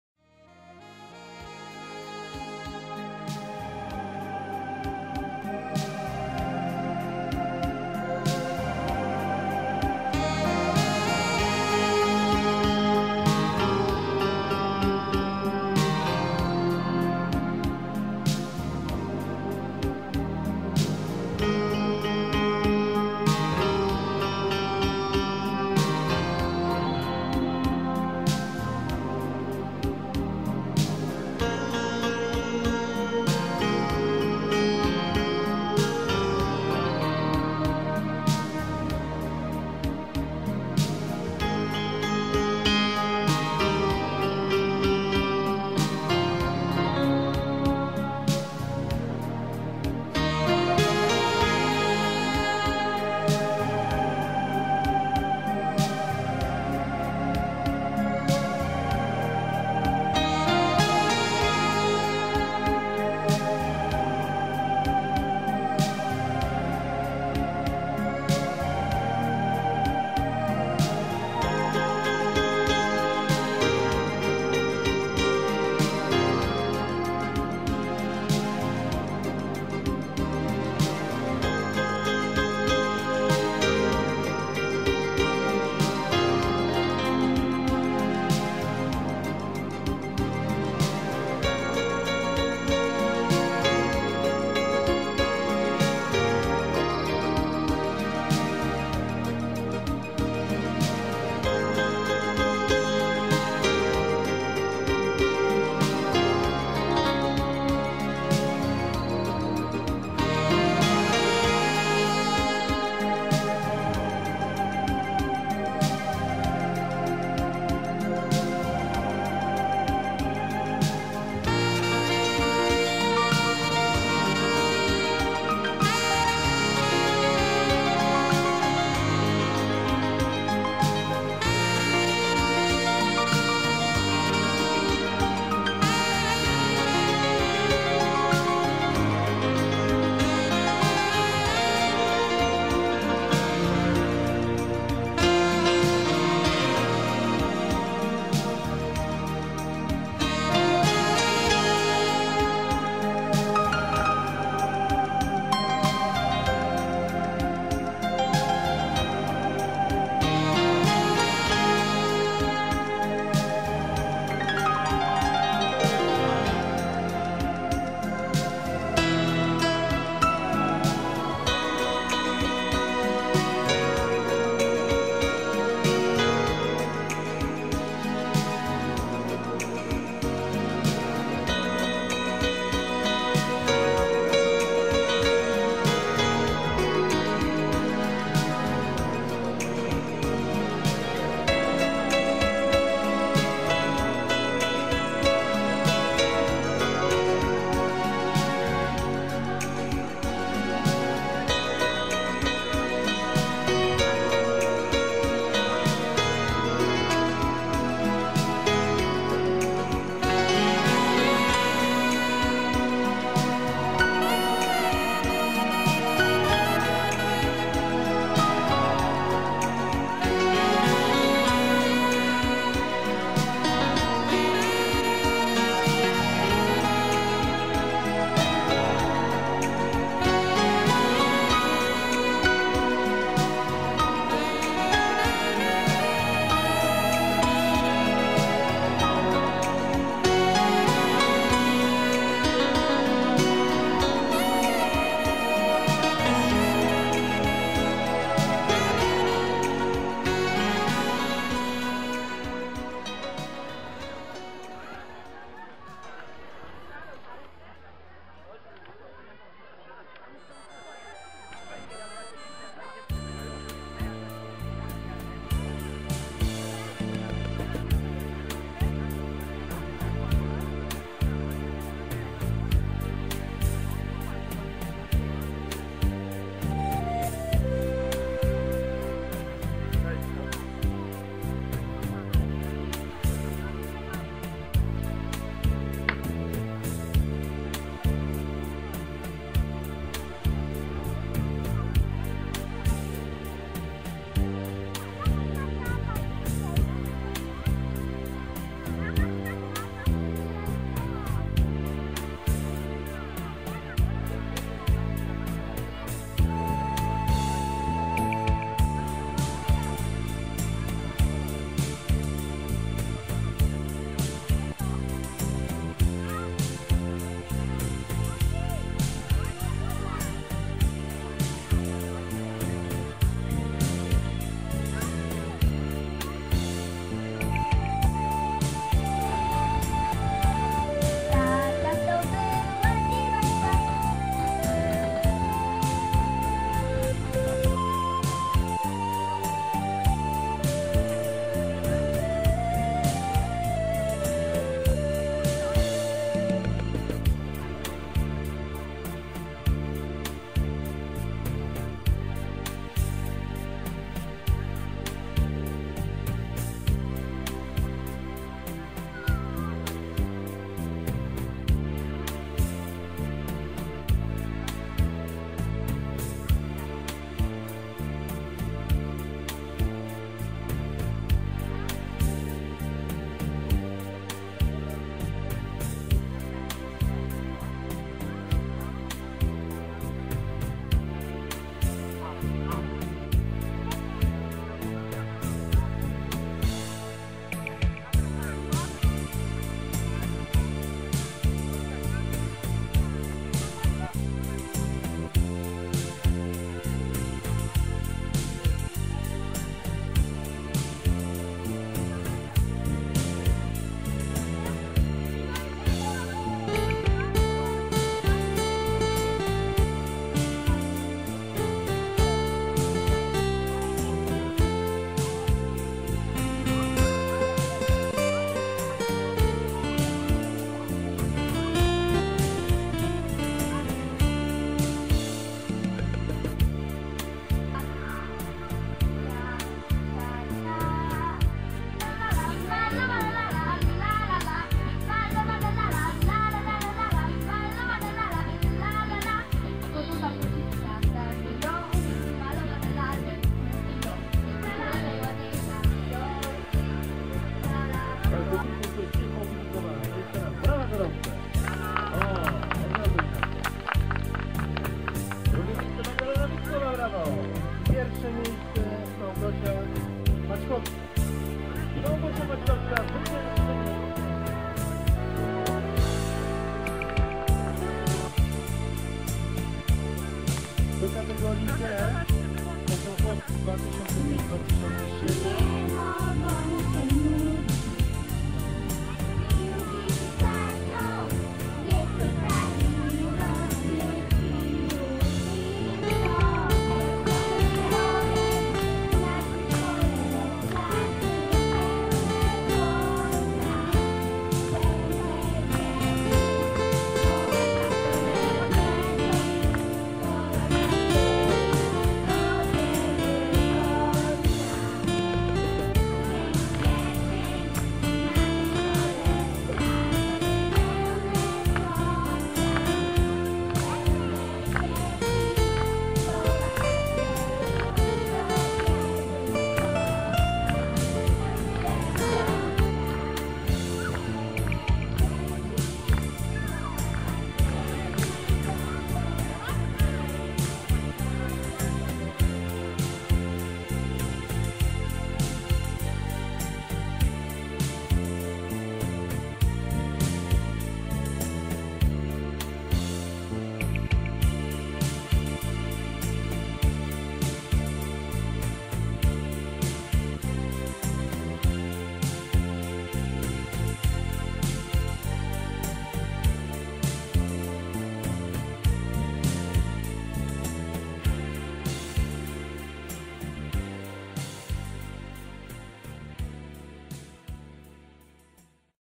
Babie Lato film
Blisko 800 osób przybyło w sobotni poranek na plac przy "Amorku" w Głuchołaskim Zdroju, gdzie zlokalizowano punkt startu VII Wojewódzkiego Rajdu Kolarsko - Pieszego "BABIE LATO W GÓRACH OPAWSKICH. Impreza zorganizowana przy współpracy, wsparciu i pomocy wielu instytucji publicznych, uczniów głuchołaskich szkół, oraz organizacji pozarządowych, a także sponsorów i osób prywatnych, adresowana była do wszystkich turystów indywidualnych i grup zrzeszonych, miłośników wędrówek rowerowych i pieszych.